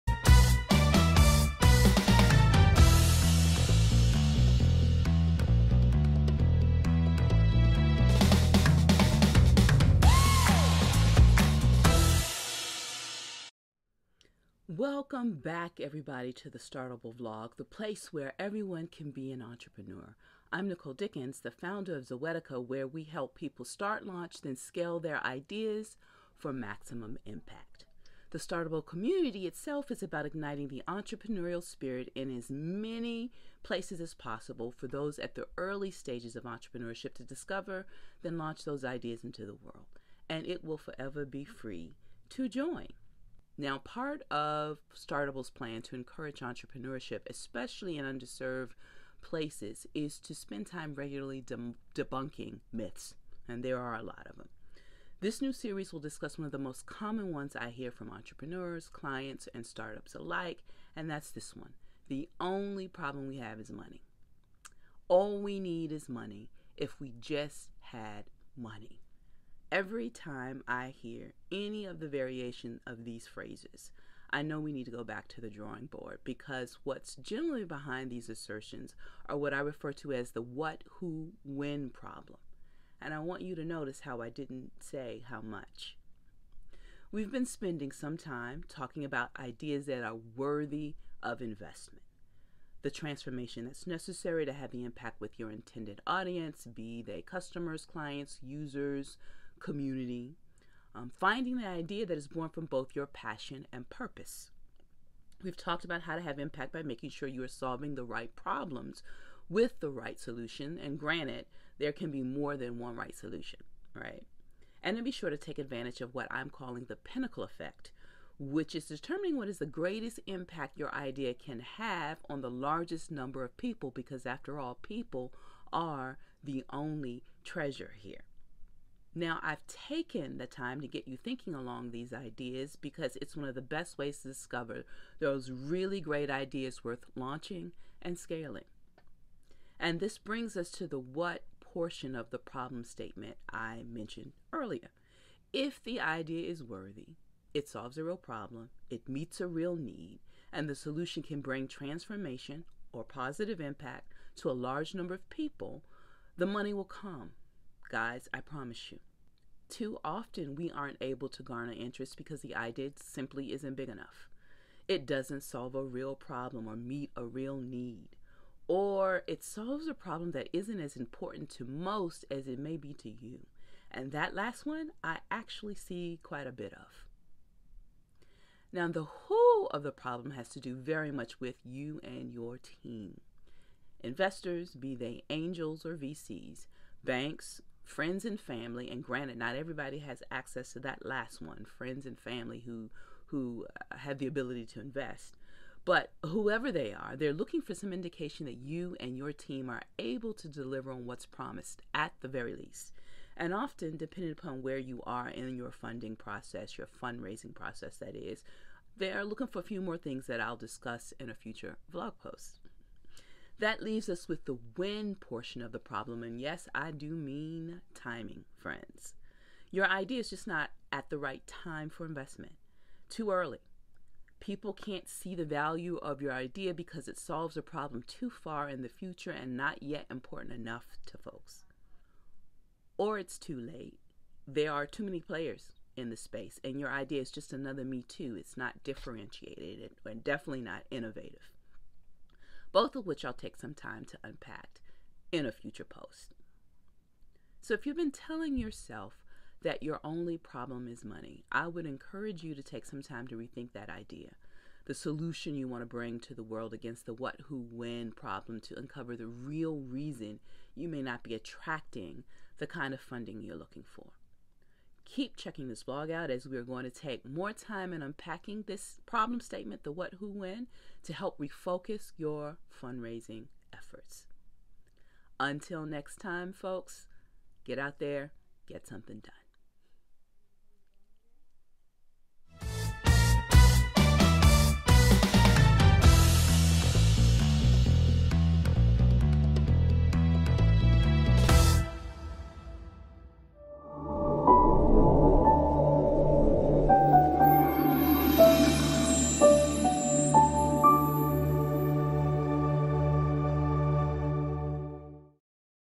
Why money is never the only problem preventing you from starting up
If you think money is your only problem when starting up, think again. Too often, entrepreneurs fail to fully understand their lack of funding is due to some pretty simple and fixable factors.